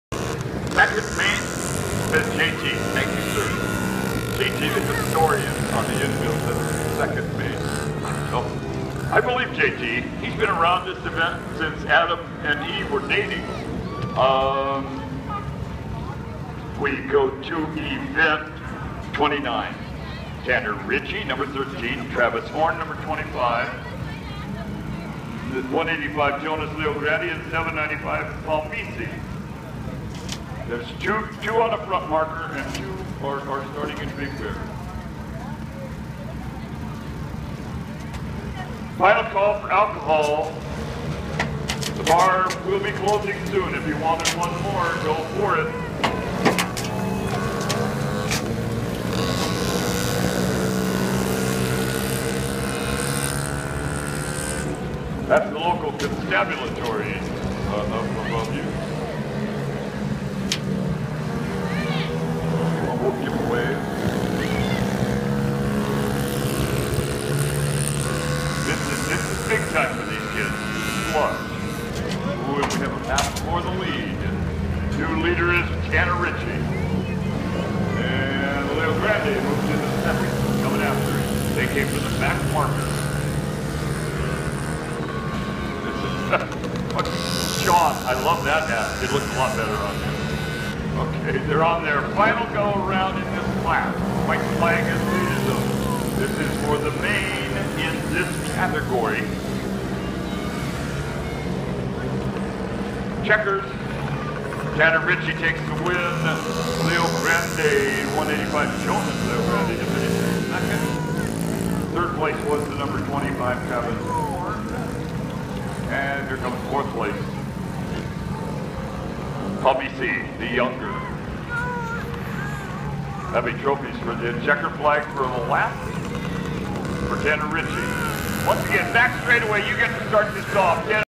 April 25, 2014 Pirate Speedway Peewee Main Event - Travis Horn
Travis Horn #25 Peewee Speedway Motorcycle Race at Pirate Speedway in San Bernardino, CA on April 25, 2014. Travis came in Third Place.